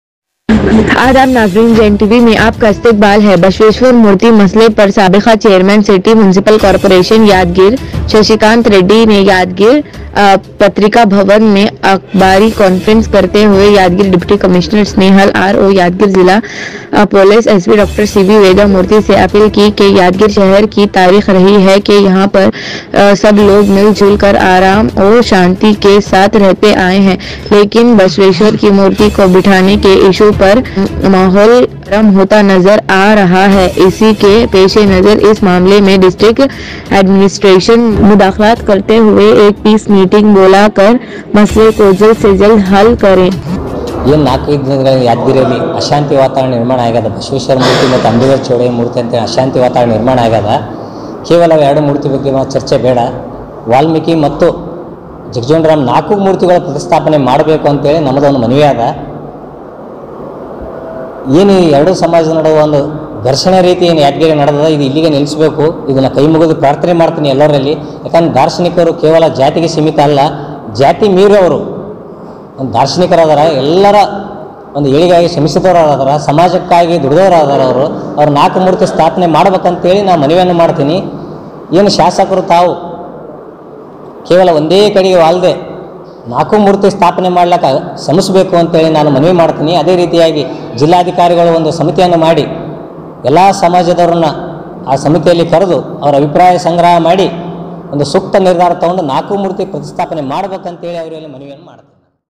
Basweshwar Murty Masle Par Sabeqa Chairman City Municipal Corporation Yadgir , Shashikant Reddy Ne Y
Basweshwar Murty Masle Par Sabeqa Chairman City Municipal Corporation Yadgir , Shashikant Reddy Ne Yadgir Patrika Bhawan Me Akbari Confrence Karte Hue Yadgir Deputy Commitioner Snehal R , O Yadgir Zila Police S P Dr C B Veda Murti , Se Apeel Ki Ke Yadgir Shaher Ki Tariq Rahi Hai Ke Yahan Par Sab Log Mil Jhul Kar Aman O Shanti Ke Saat Rahete Aye Hain , Lekain Basweshwar Ki Murty Ko Bithane Ke Issu Par Mahol Garam Hota Nazar Aa Raha Hai , Isi Ke Pesh Nazar Is Mamle Me District Administration Mudaqlat Karte Hue Ek Piece Meeting Bola Kar Masle Ko Jald Se Jald Hal Kare ,